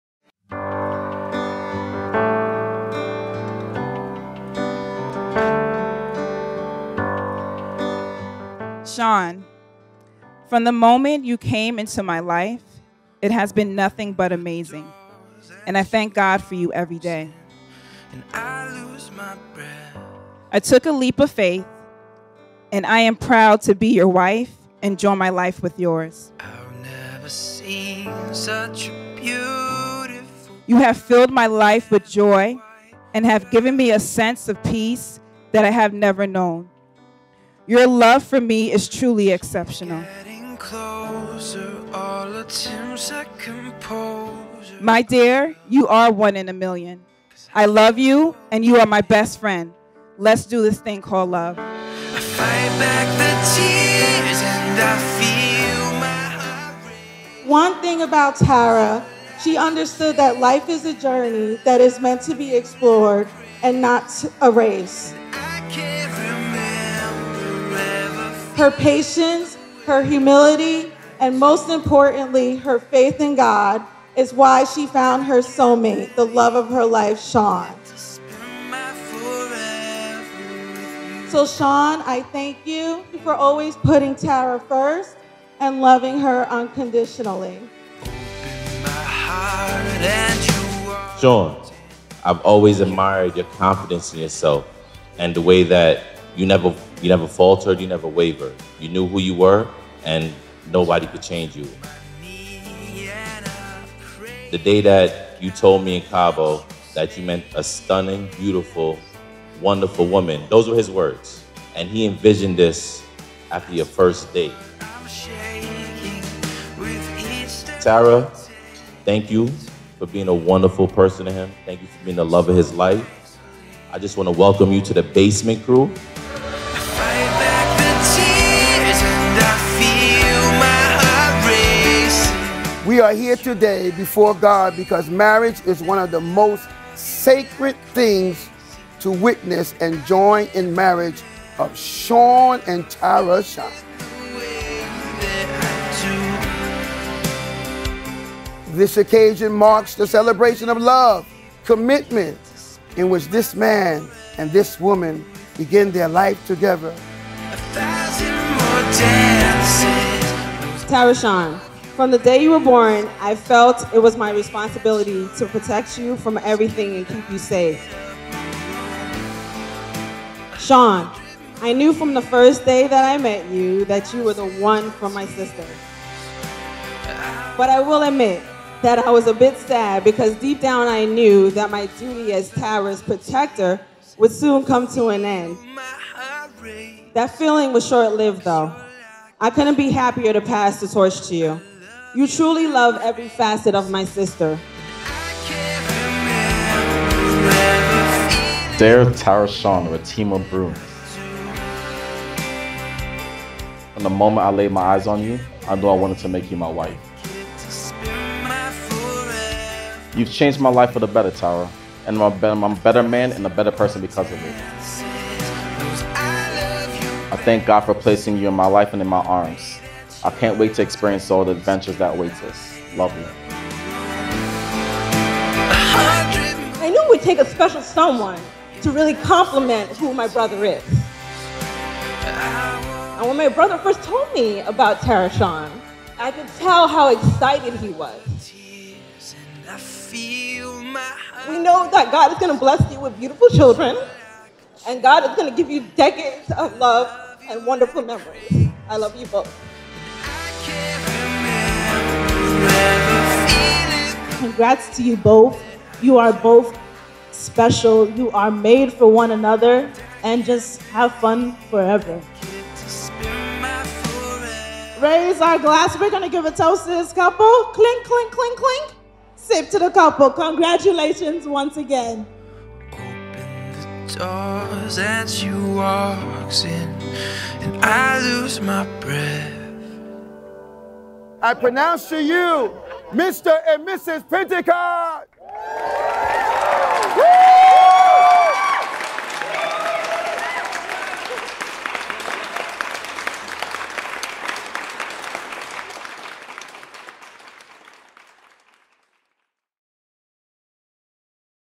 Tarashon and Shaun Wedding Highlight at The Terrace, NJ
Tarashon & Shaun are such a wonderful couple! They are true soul mates. The love they have for each other shines through their eyes and inspires everyone around them. We were so happy to have played part in their special day. Together with their closest family and friends, these two love birds sealed the knot during a breathtaking ceremony at The Terrace, NJ and later danced away surrounded by their loved ones. Congratulations Tarashon & Shaun, we wish you a lifetime of happiness together!